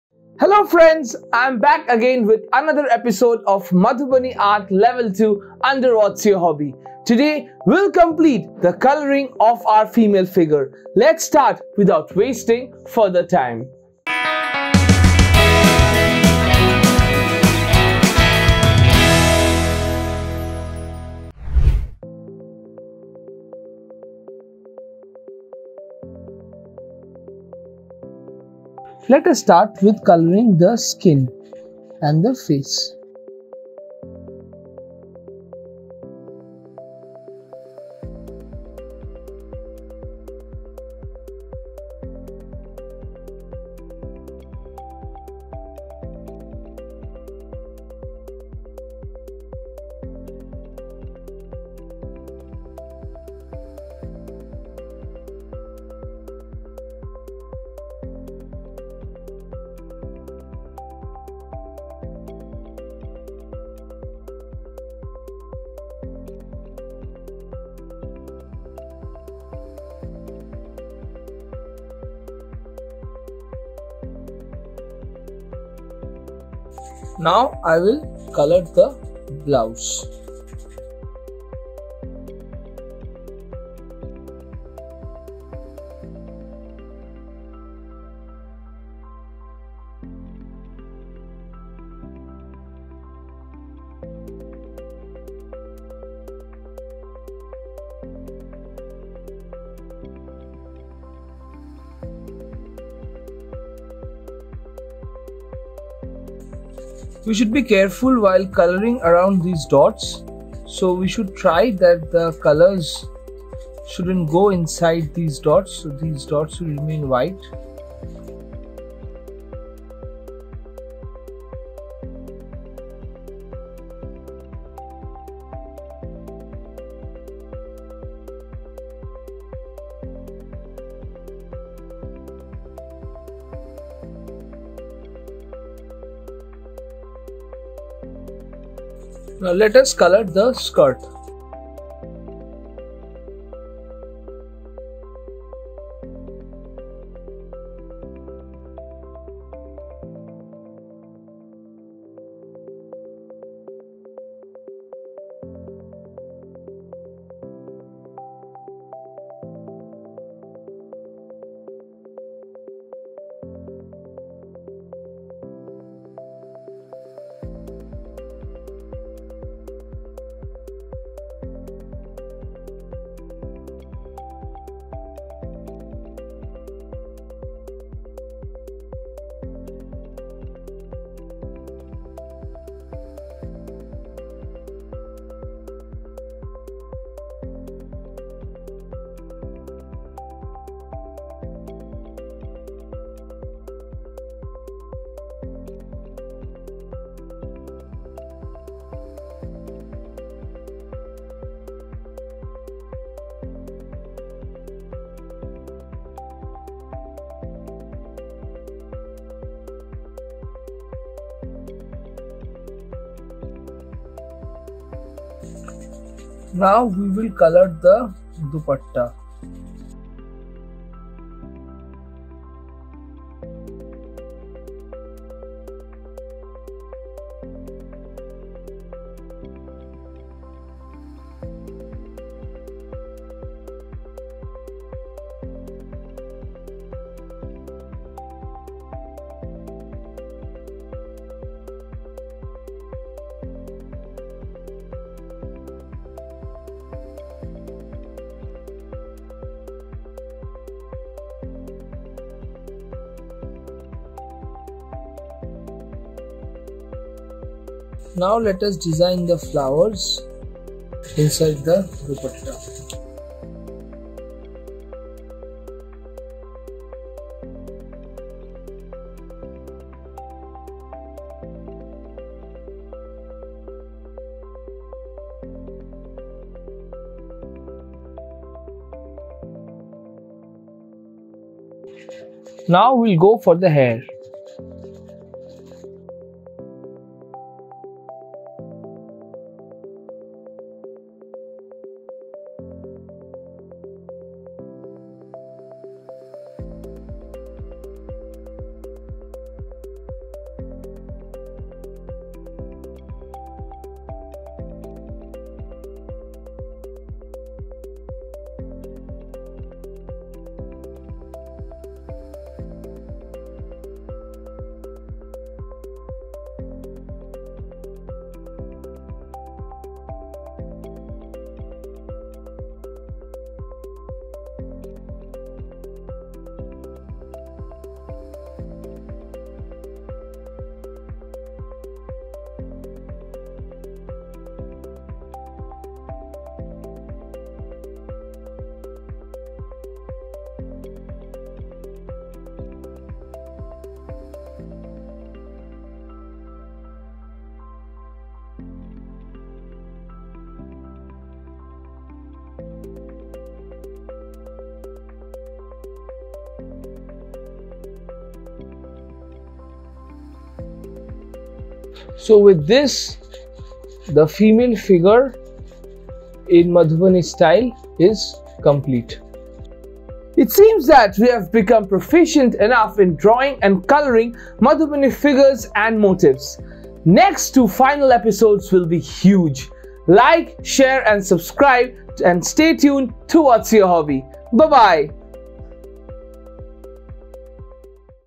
Female Figure in Madhubani | Part 2 | Tutorial for Beginners
Hey Beautiful people. Welcome to another episode  of Madhubani Art Level 2 by  "What is Your Hobby?" It is time to colour the sketch of the female we made in our previous episode.